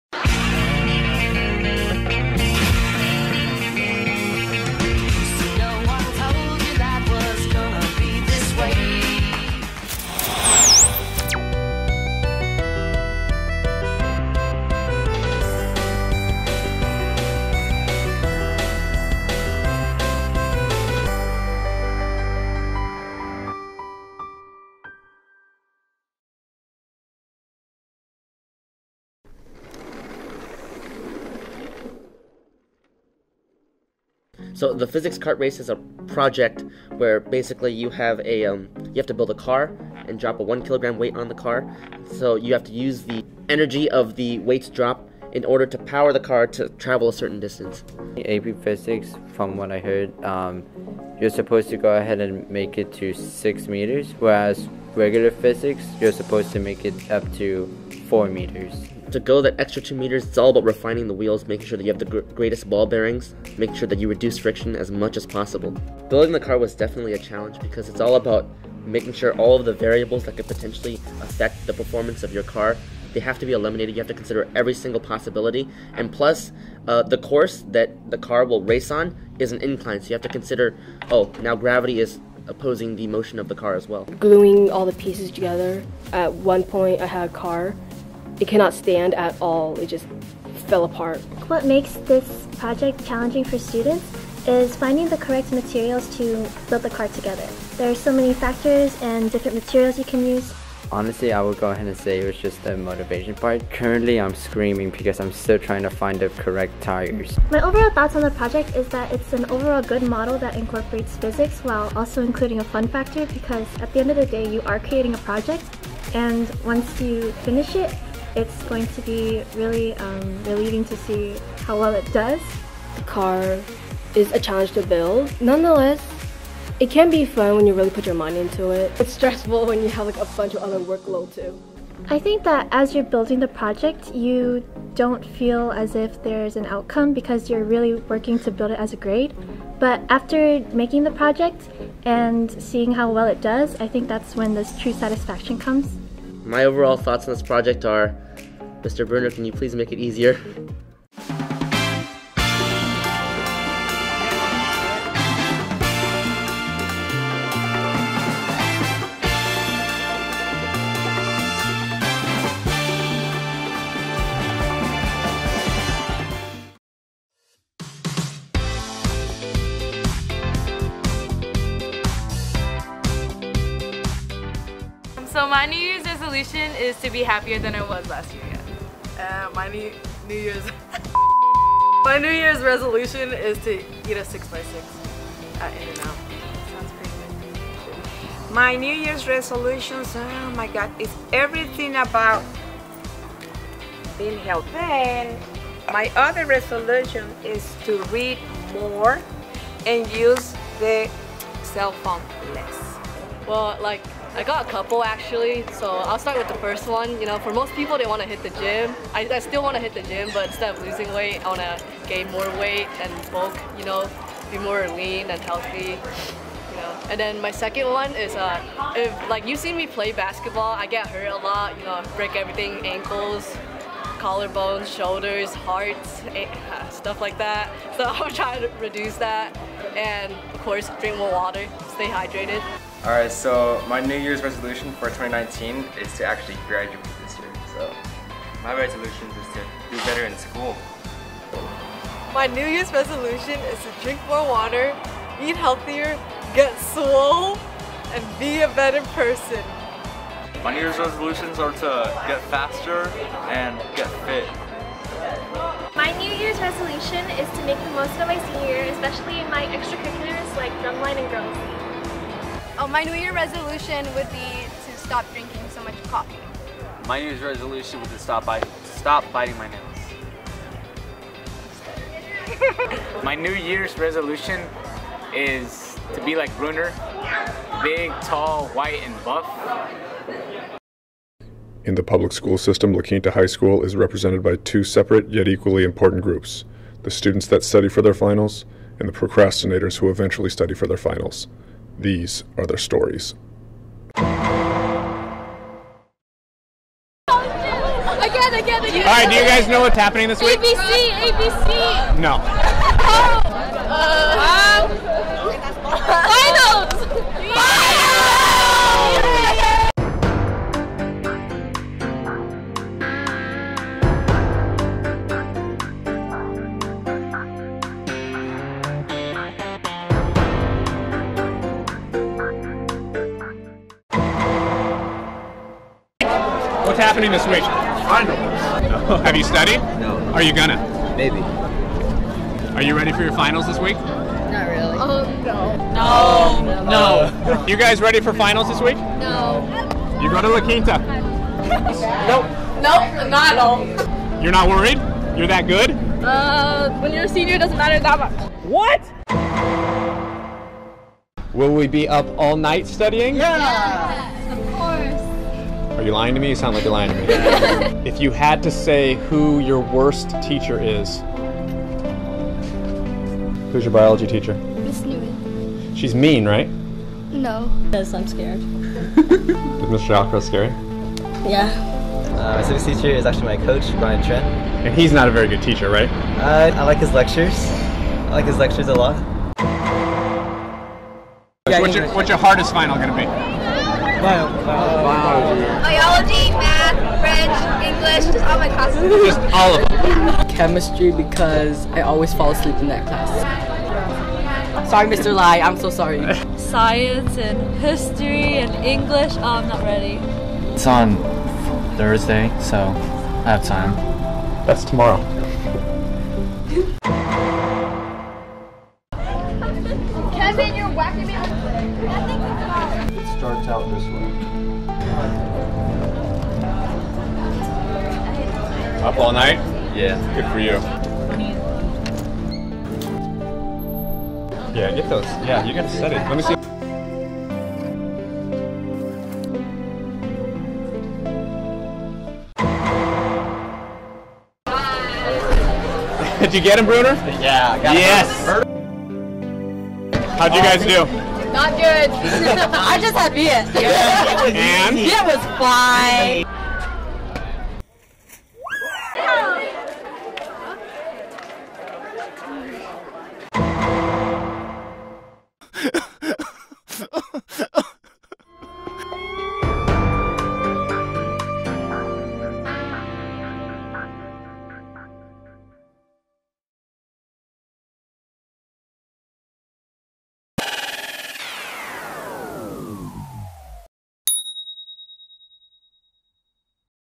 S1Ep5 ABC Broadcast [February]
This is the Aztec Broadcast Channel's January 2019 Broadcast.

This month's segments include:
1. Physics Cart Race
2. New Years Resolutions
3. 1st Semester Finals

To see more videos from Mr. Ruzzi's Digital Media classes,